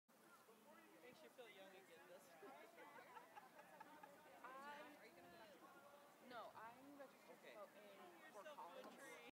Supporters line up outside CSU for Bernie Sanders
Bernie Sanders supporters line wrapped around CSU hours before the 7 p.m. rally started.